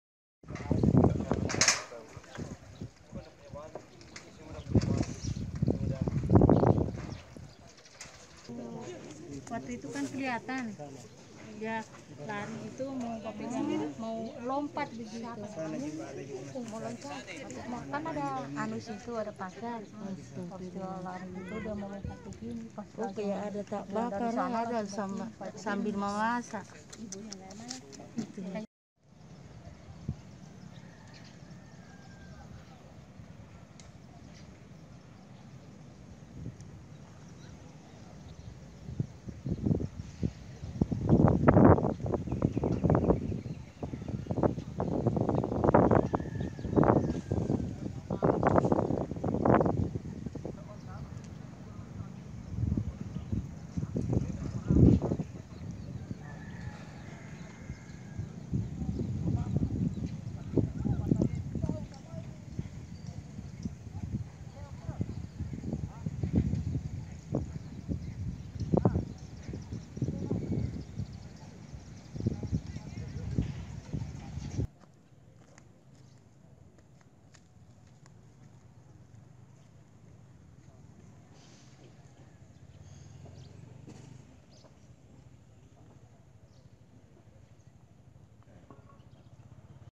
Homes Shattered By Indonesia Earthquake-Tsunami
September 30, 2018 - Buildings and homes are destroyed by an Indonesian quake-tsunami that has killed at least 830 people. IMAGES